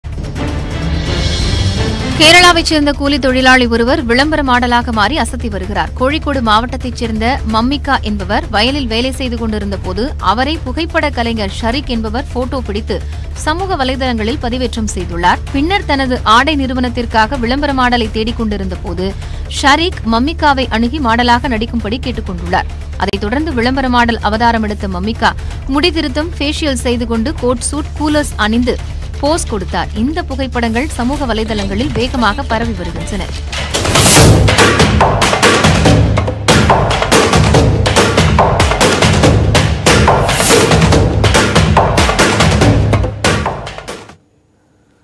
Kerala Model Mammika : இப்படி இருந்த நான் இப்படி ஆயிட்டேன் - கூலி தொழிலாளி டு சூப்பர் மாடல்
இப்படி இருந்த நான் இப்படி ஆயிட்டேன் - கூலி தொழிலாளி டு சூப்பர் மாடல் | Kerala Model Mammika